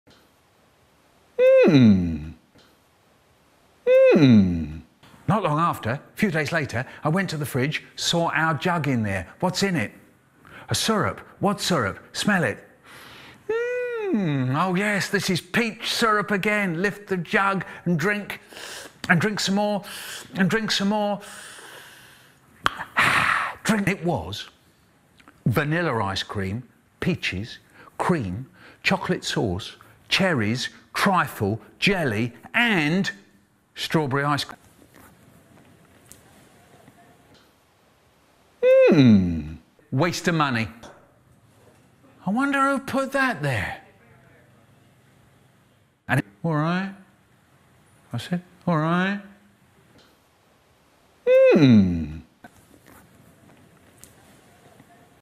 Michael Rosen describes People can Fly games
They are devs of Painkiller, BulletStorm, Gears Judgement and the Outriders Series.